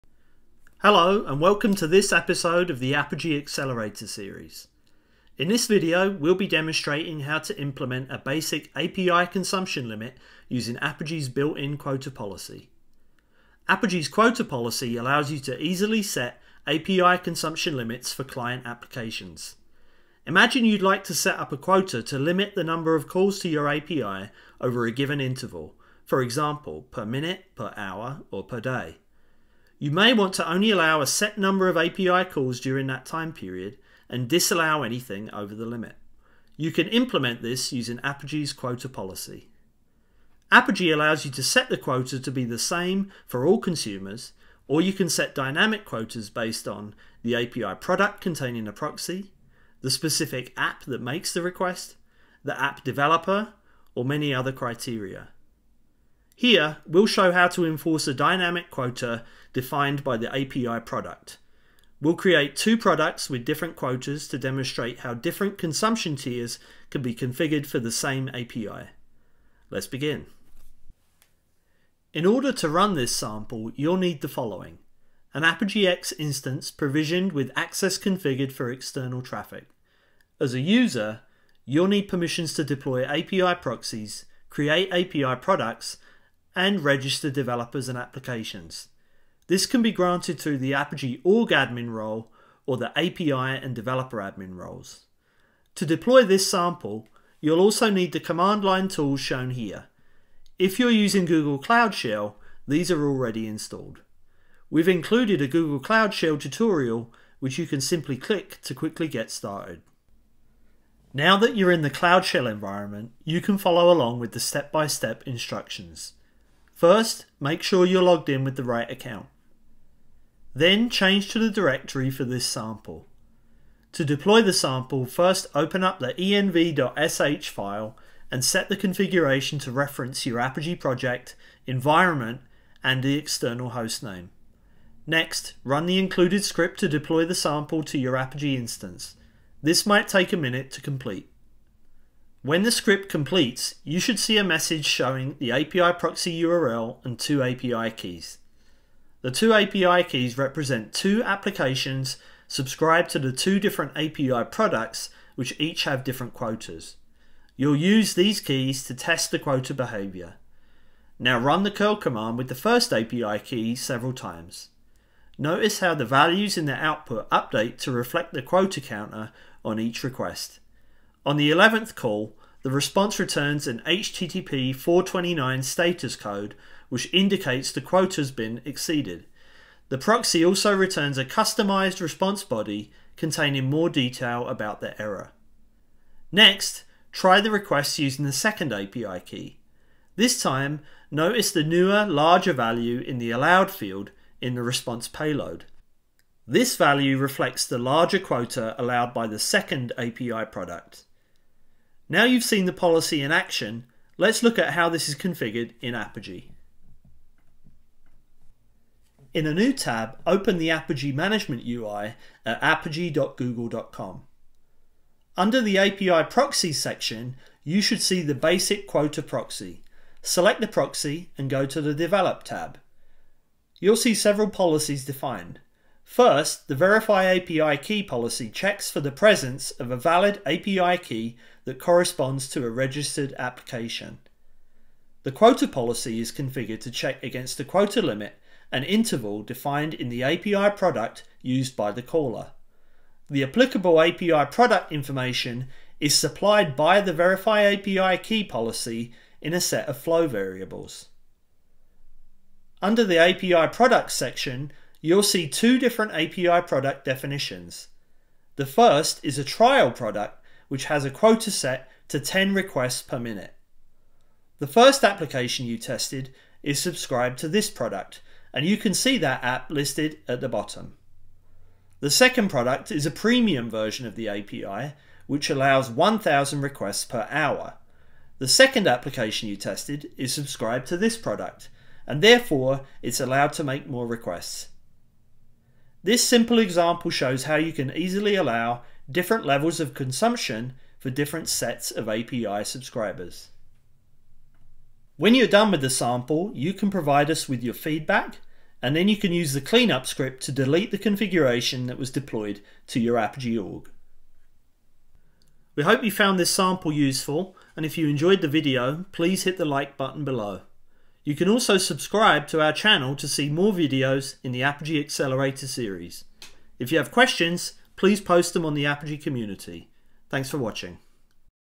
Implementing Dynamic Quotas with Apigee
In this video, you'll learn how to implement a dynamic API consumption limit using Apigee's Quota policy.